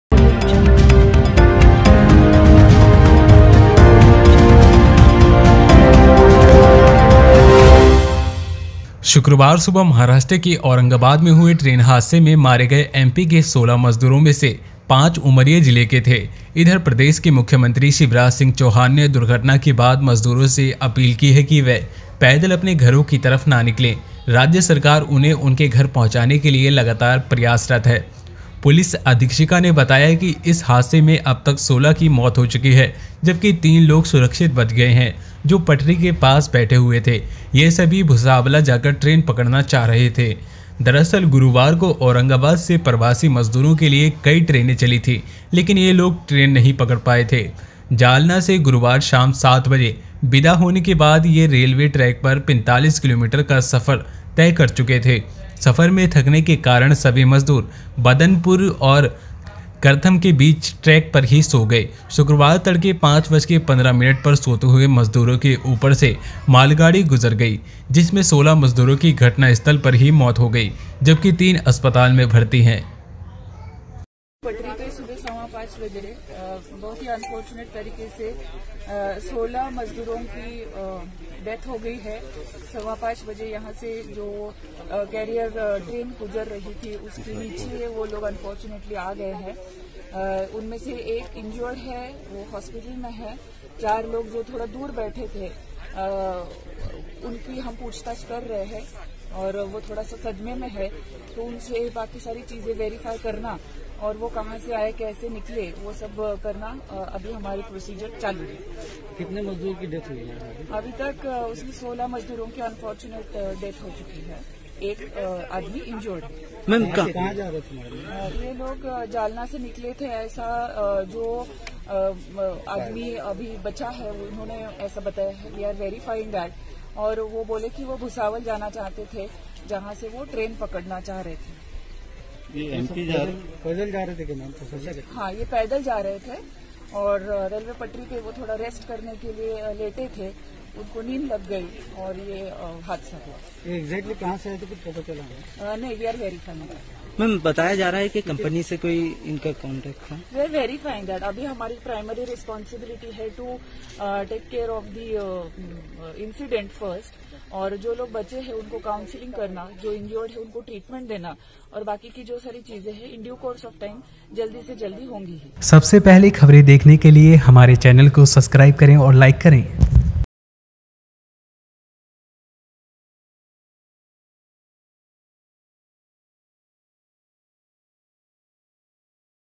महाराष्ट्र: औरंगाबाद में ट्रेन से कटकर 16 मजदूरों की मौत, मृतकों के परिजनों को 5 लाख के मुआवजे का...
महाराष्ट्र: औरंगाबाद में ट्रेन से कटकर 16 मजदूरों की मौत, मृतकों के परिजनों को 5 लाख के मुआवजे का एलान